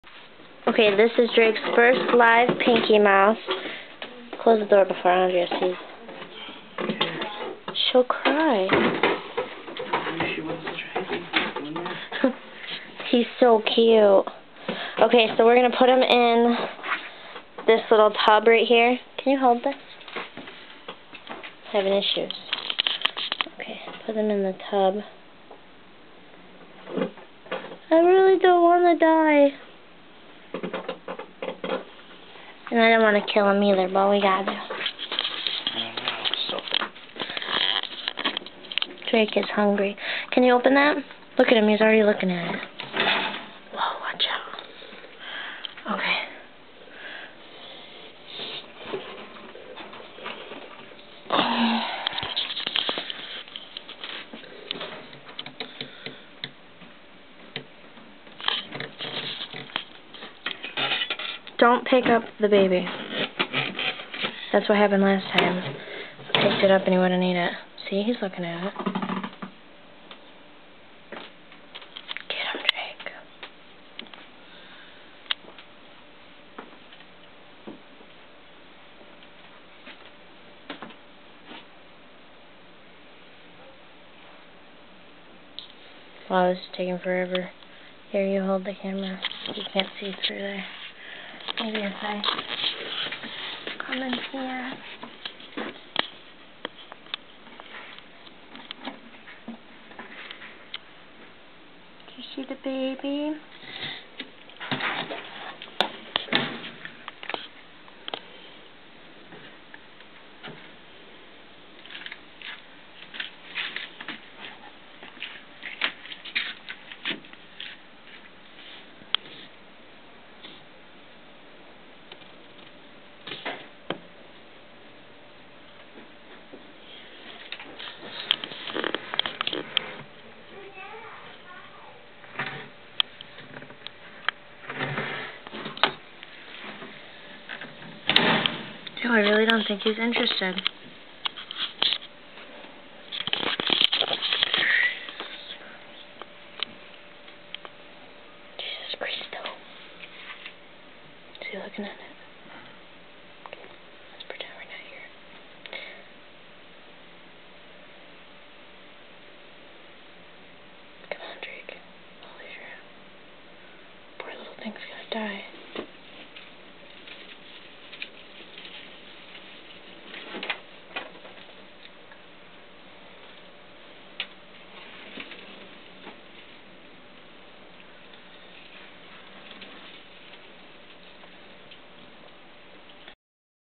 pinky
The Pinky. Very cute but he was food. Kinda sad. He loved me lol. Took him out and he started suckling on my hand tryin to get milk. Very adorable.